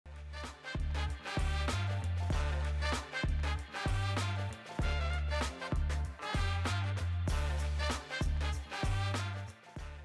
John Dory's brings you The Cozy Combo!
Stay cozy with the John Dory’s Cozy Combo of 2 hake fillets and 5 JD’s prawns brushed in our famous basting, served with chips and rice for only R139 - add a glass or bottle of Laborie wine and stand a chance to win 1 of 10 R10k shopping vouchers!